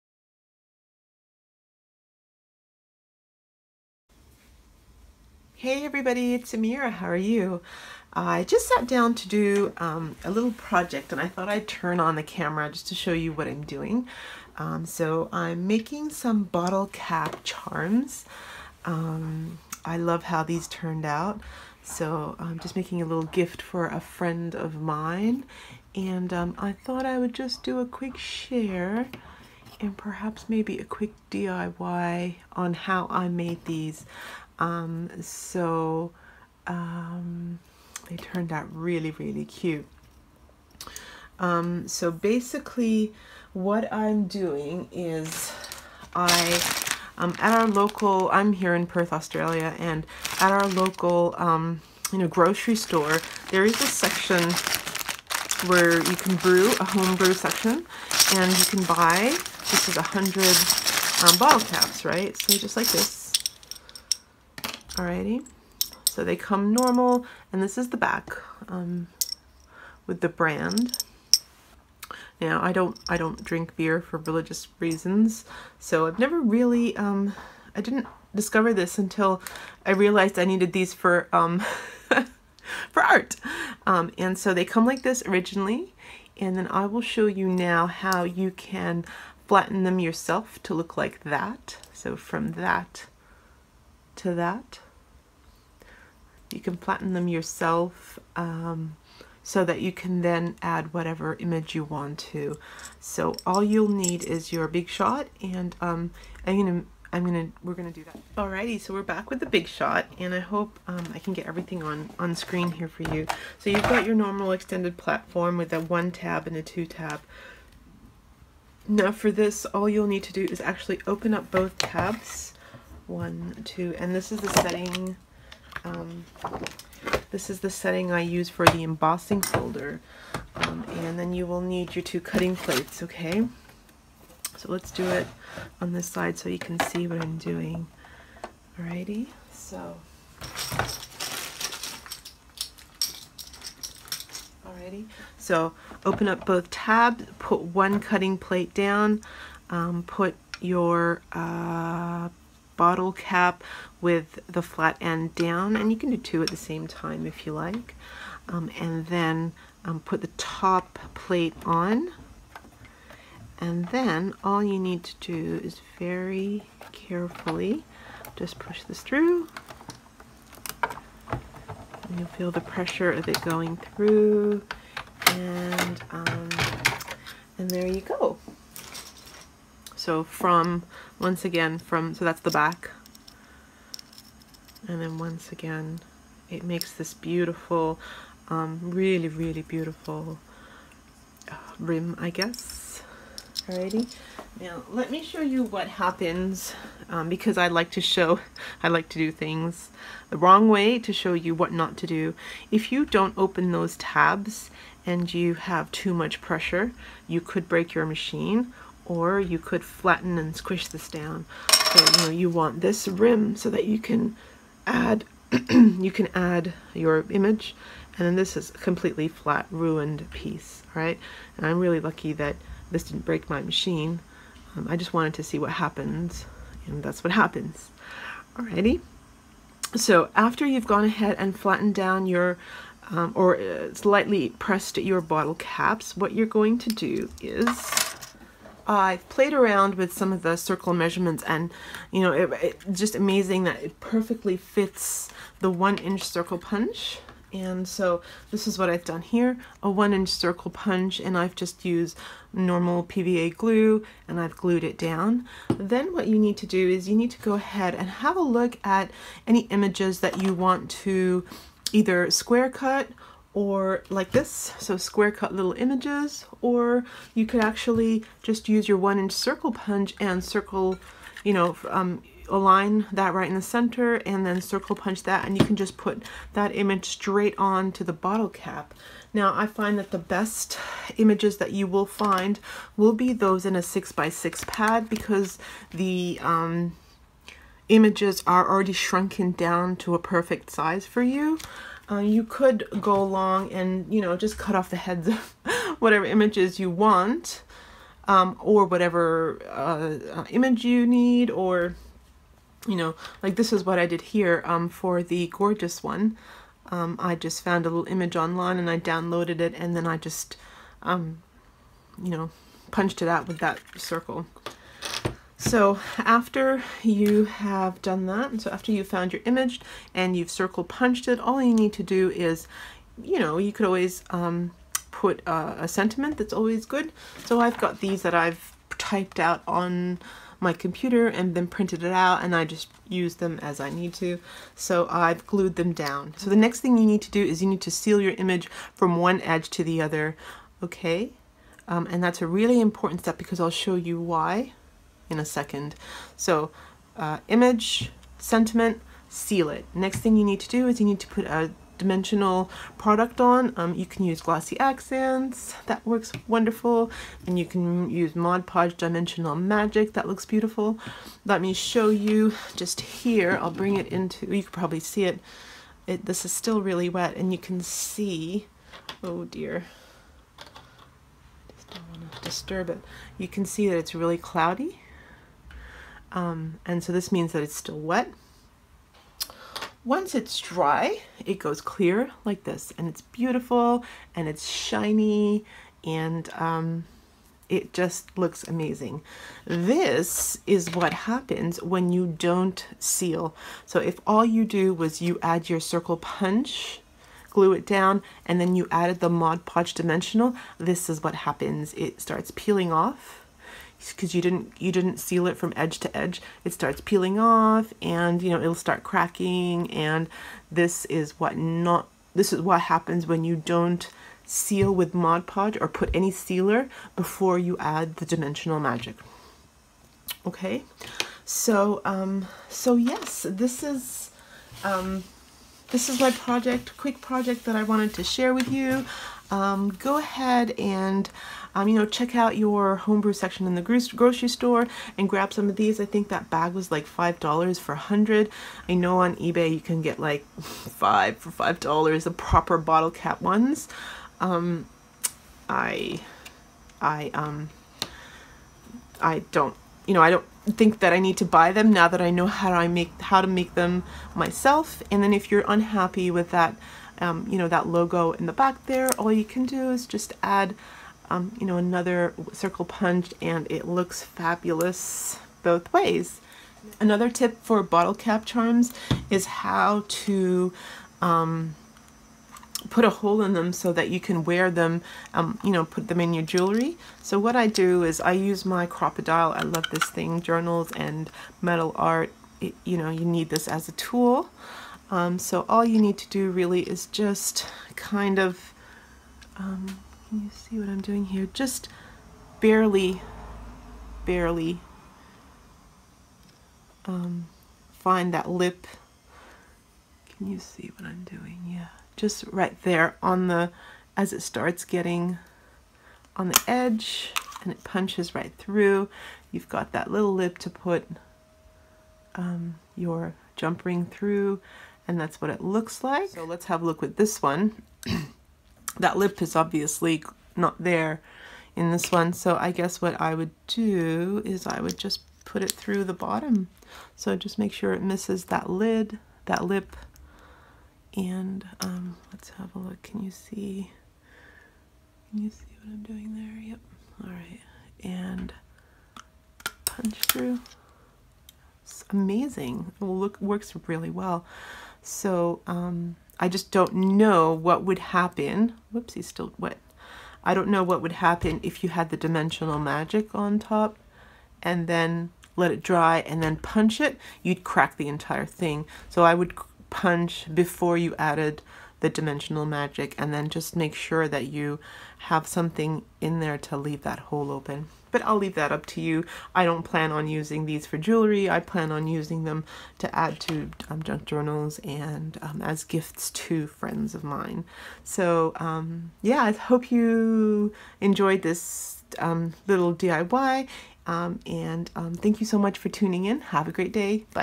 DIY Bottle Cap Charms Tutorial
Watch how I make my own bottle cap charms using bottle caps I bought from the local store and dimensional product.